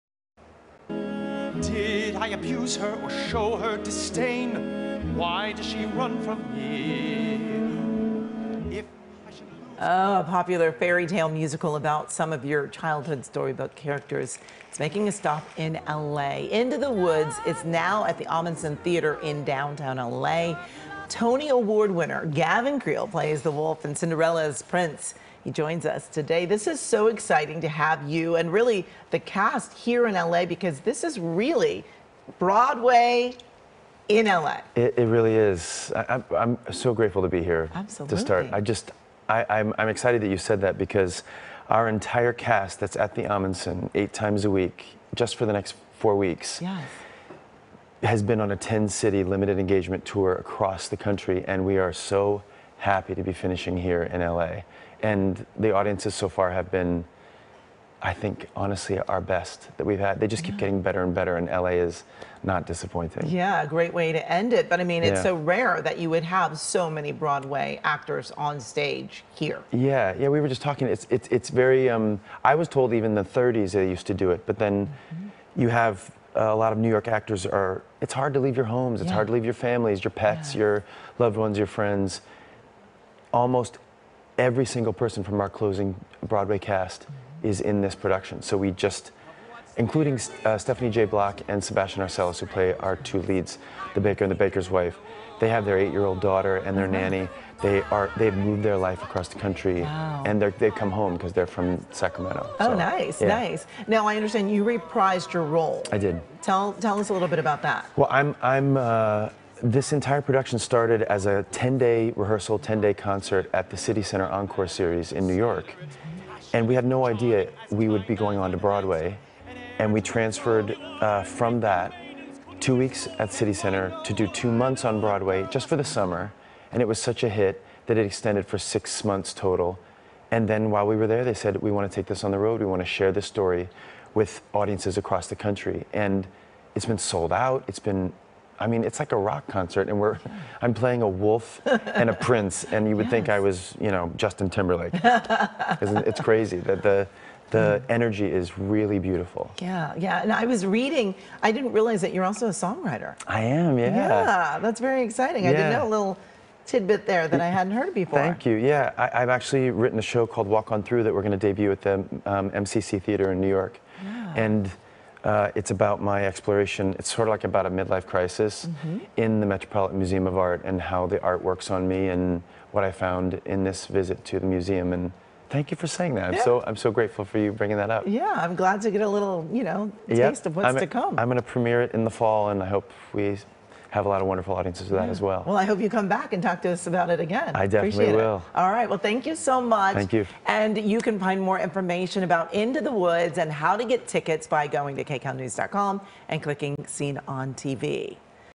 Gavin Creel from "Into The Woods"
"Into The Woods" is now at the Ahmanson Theater and Gavin Creel, who plays Cinderella's prince (and the wolf) , was here to talk about the show, which runs through July 30th!